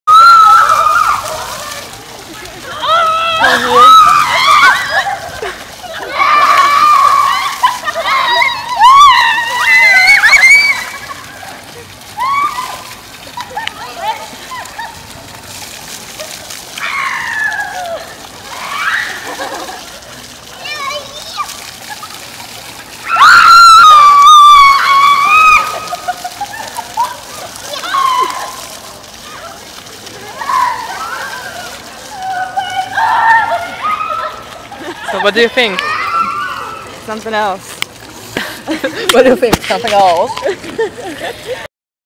Girls Playing Around in the Fountain
Random girls playing with fountain in front of the Vintage mall, Modesto, California. So spontaneous.